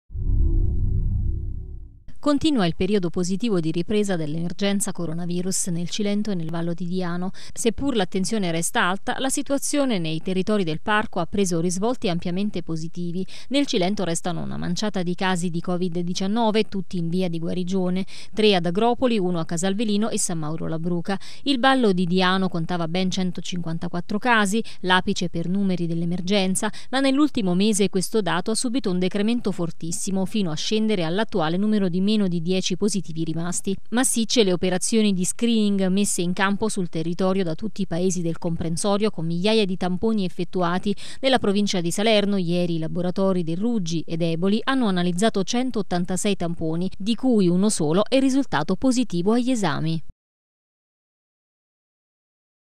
Cilento e Diano, si riprende dall'emergenza a piccoli passi l'area del Parco
Continua il periodo positivo di ripresa dall’emergenza Coronavirus nel Cilento e nel Vallo di Diano. Seppur l’attenzione resti alta, la situazione nei territori del Parco ha preso risvolti ampiamente positivi.